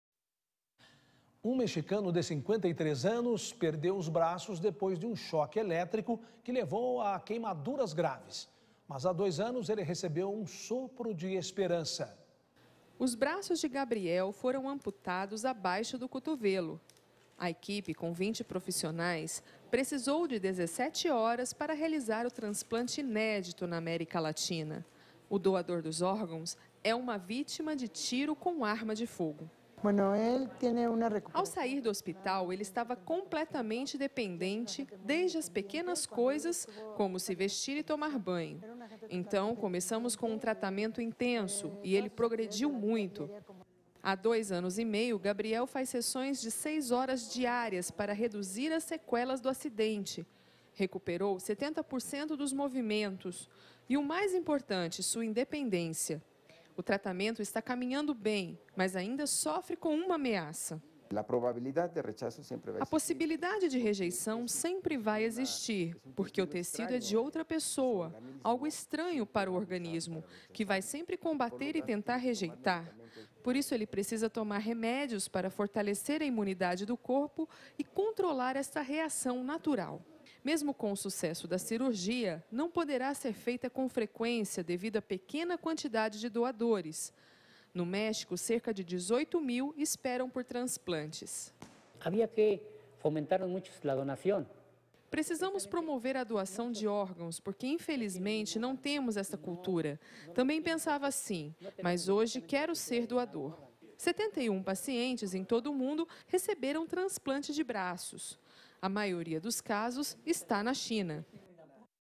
CN Notícias: Mexicano ganha transplante de braços - 28/11/13
Um mexicano, de 53 anos, perdeu os braços depois de um choque elétrico que levou à queimaduras graves. Mas há dois anos ele recebeu um sopro de esperança. *Leve o jornalismo Canção Nova para o seu celular